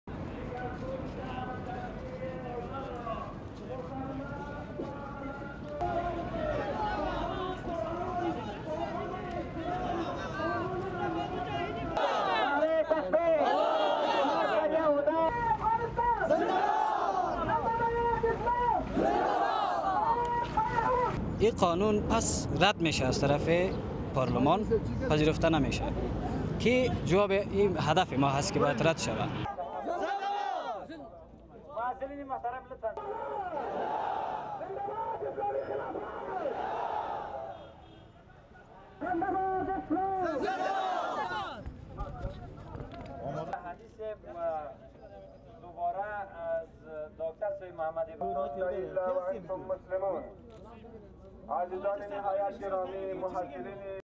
Hardline students demand the repeal of women's rights law
1. Wide of police and protesters
2. Various of protesters chanting UPSOUND: (Dari) "Long live Islam, long live Afghanistan, death to infidels"
3. SOUNDBITE: (Dari) Mawladad Jalali, mullah of Kabul University mosque and protest organiser
"This law should be repealed in parliament, and should not be accepted. This is our main demand."
4. Mid of protesters chanting UPSOUND: (Dari) "Long live Islam"
5. Close of banner showing Holy Quran
6. Mid of protesters chanting UPSOUND: (Dari) "Long live Islam"
7. Close of Islamic banner
8. Wide of protesters
STORYLINE:
Hard-line Islamic students protested in Kabul on Wednesday, demanding the repeal of a presidential decree for women's rights that they say is un-Islamic.
It was the latest sign of a backlash against the legal protections passed in the 12 years since the toppling of the Taliban regime known for its harsh treatment of women.
More than 200 male students protested in front of Kabul University on Wednesday against the decree on Elimination of Violence Against Women, which includes a ban on child marriage and forced marriage, makes domestic violence a crime and says rape victims cannot be prosecuted for adultery.
It also outlaws "ba'ad," a traditional practice of exchanging women or girls to settle disputes or debts.
Wednesday's protest came days after conservative lawmakers' vehement opposition blocked an attempt to cement the decree's provisions in legislation.
Mawladad Jalali, the mullah of the university mosque and one of the organisers of the protest, led chants decrying democracy in general and the women's law specifically.
"This law should be repealed in parliament, and should not be accepted. This is our main demand," he said.
Most of the international force that ousted the Taliban is now preparing to withdraw by the end of next year, and activists fear an erosion of the women's rights will follow if hard-liners pressure the elected government.
Afghan President Hamid Karzai issued the decree on women's rights three years ago as part of a raft of commitments to international donors, but lawmaker and activist Fawzia Kofi wanted to pass it in parliament to prevent any future president from reversing it.
A brief parliamentary debate on Saturday was ended by the speaker after fierce opposition from conservative lawmakers who said several provisions - including the ban on child marriage and jail time for domestic abuse - violated Islamic law.
The decree remains in force, but the debate appears to have galvanised opposition to it.
The United Nations' mission in Afghanistan this week urged the government to do more to enforce the women's rights decree, saying it is only sporadically applied when women report abuse.
In another worrisome sign for activists, the international group Human Rights Watch said on Tuesday that the number of women and girls jailed for alleged loose morals is the highest since the ouster of the Taliban, even though most of those detained are victims of abuse and have committed no crime under Afghan civil law.
The Taliban imposed their strict interpretation of Islamic law during their five-year reign, ordering beatings for women failing to wear the full-body burqa garment in public, closing girls' schools and banning women from leaving their homes without a male relative.
They were toppled in a US-led invasion for sheltering the leadership of al-Qaida and continue to wage an insurgency.